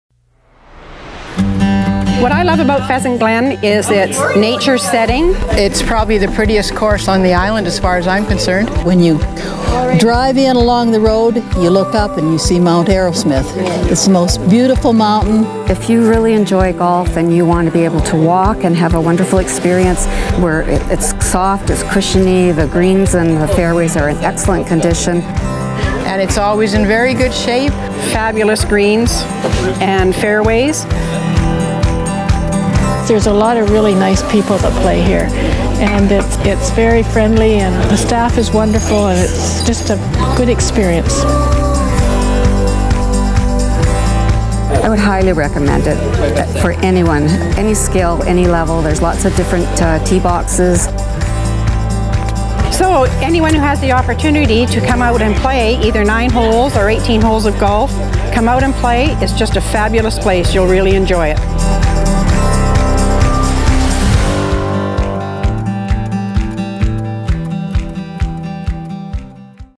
Pheasant Glen Golf Course Reviews Vancouver Island Golf Courses
Pheasant Glen Golf Resort is located in beautiful Qualicum Beach, Vancouver Island, British Columbia, Canada

See why the golf course reviews consider Pheasant Glen to be among the Best Vancouver Island Golf Courses.